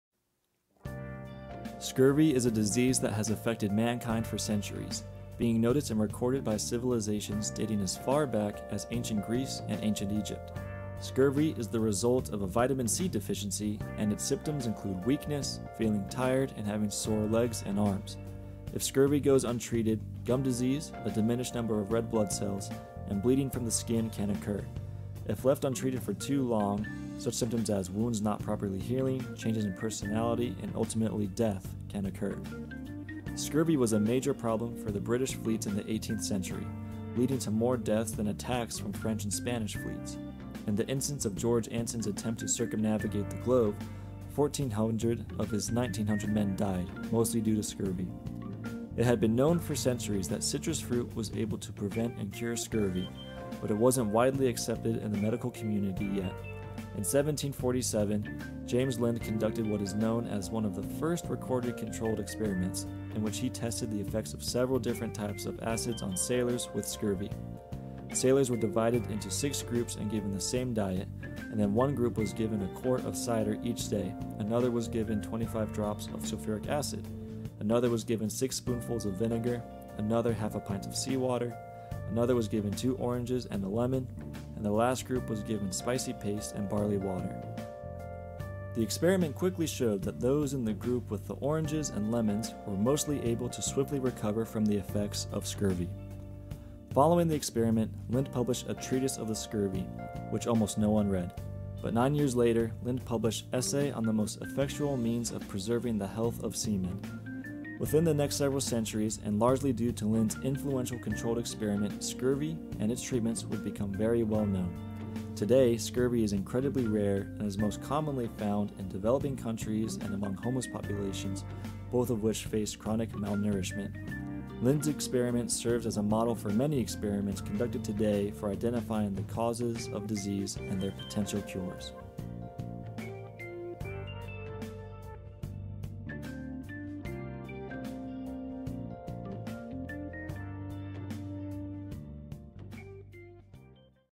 James Lind Opponent of Scurvy
Video project for my Public Health class at BYU! Give it a view!